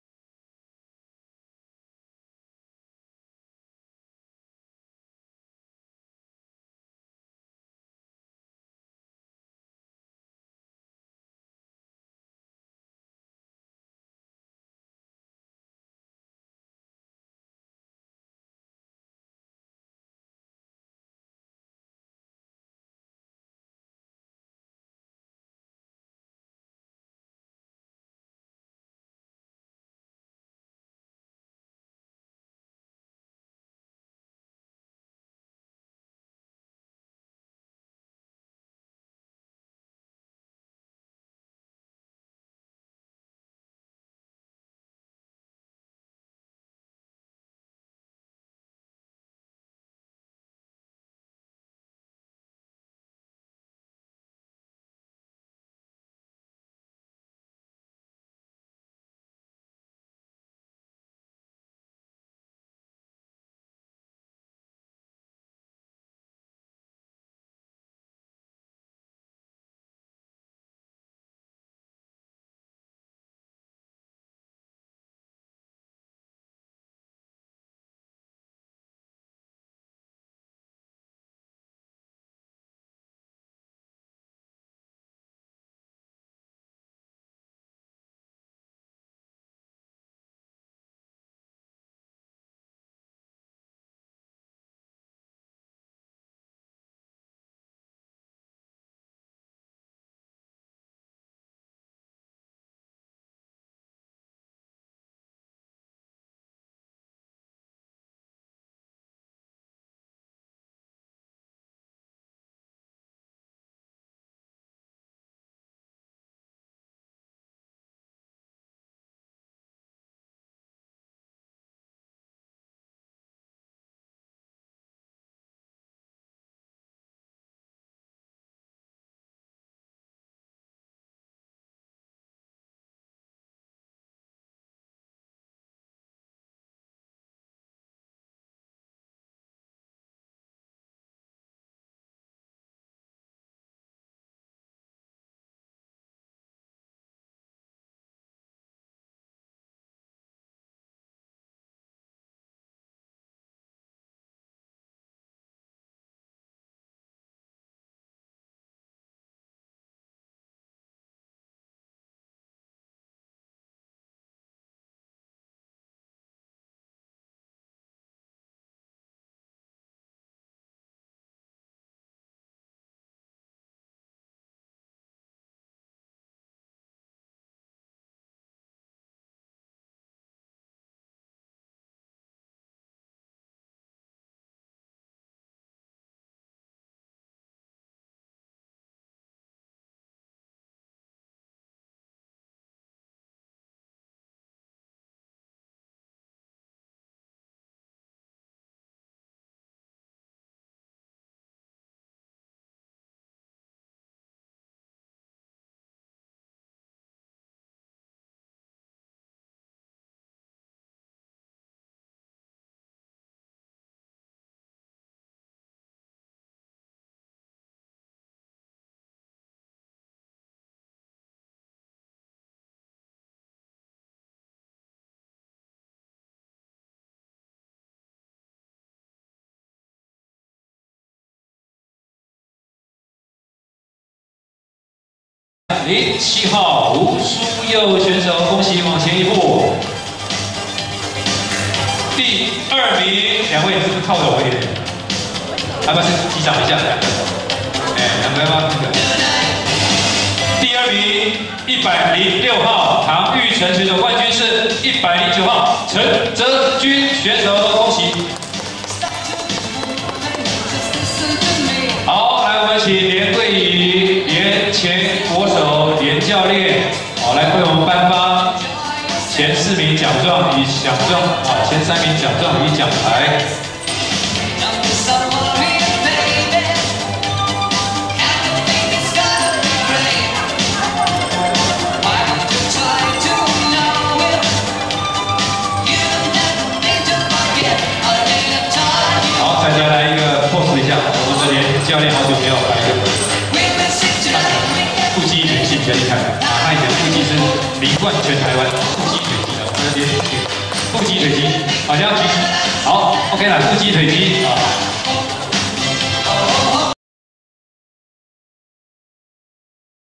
【鐵克健身】2021 台灣先生盃 健美 -70kg, -75kg Men's Bodybuilding
*為避免比賽音樂遭到YT版權審核而噤聲或下架，影片必須靜音。不再另行配樂。